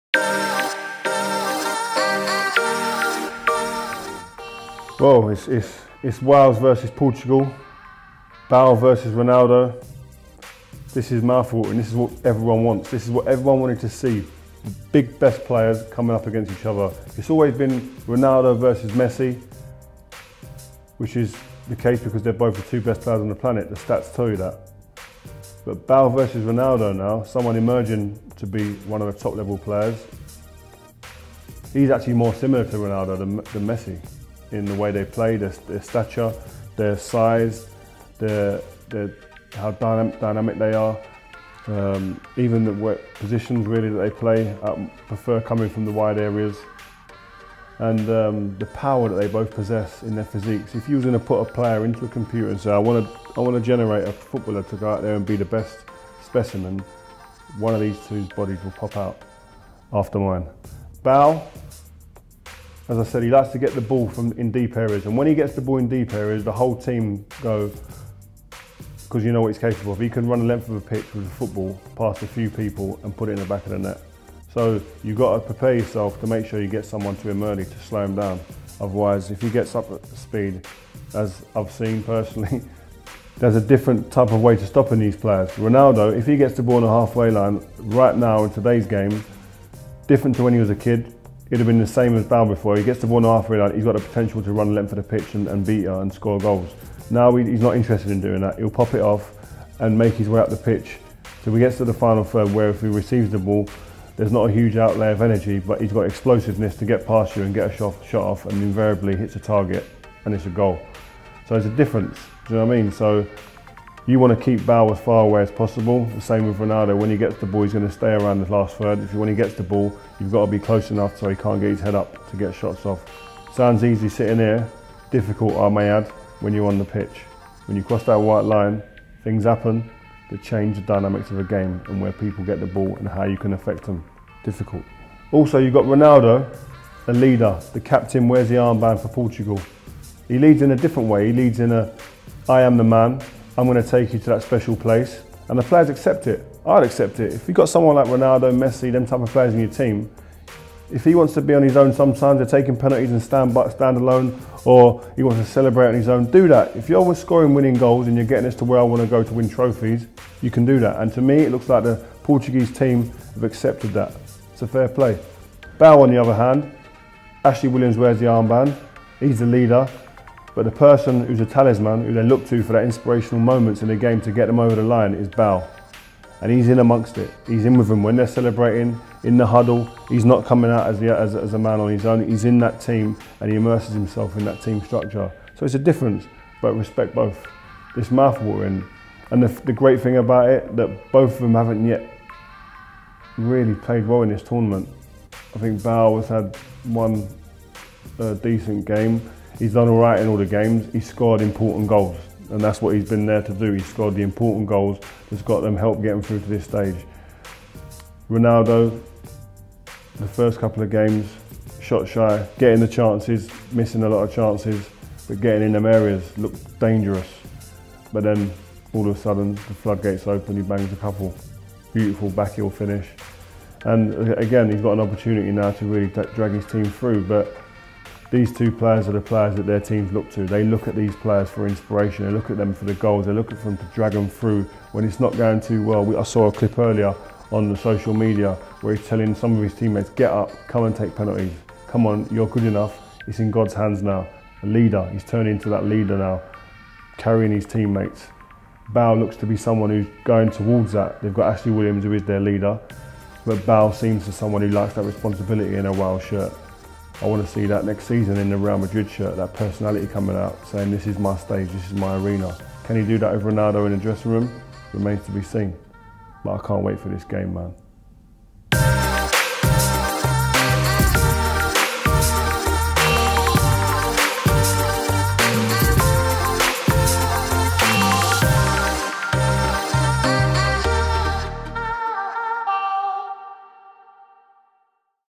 Wales vs Portugal.. Bale vs Ronaldo Here's Rio's Thoughts
Rio gives us his opinion on the forthcoming epic battle between Cristiano Ronaldo's prolific Portugal and Gareth Bale's mighty Wales. Who will win?